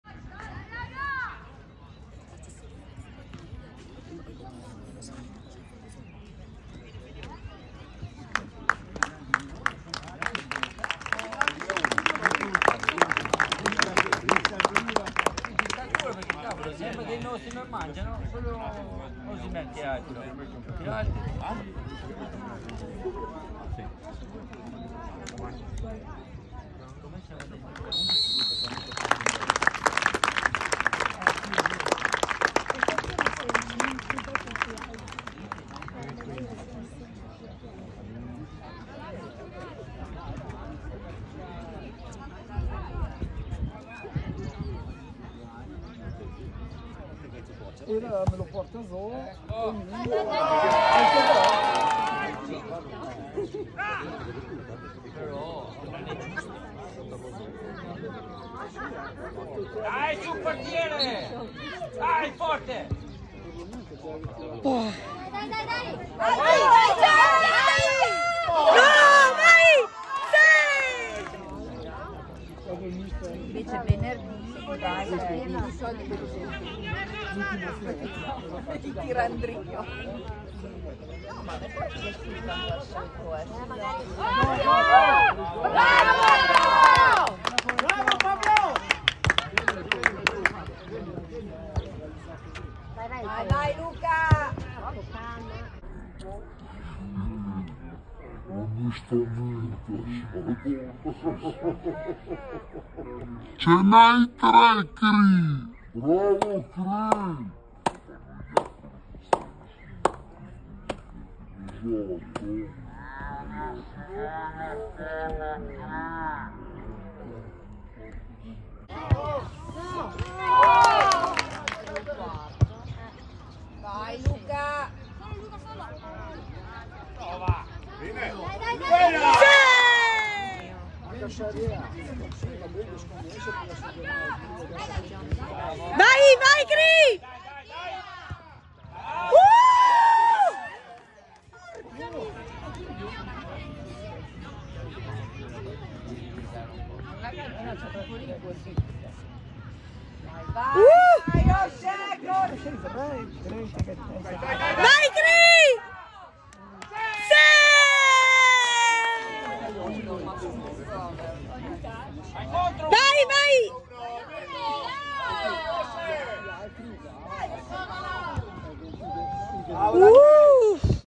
Benacense 2- Mori 5
Quarta partita di campionato, quarta vittoria per i ragazzi del Mori Santo Stefano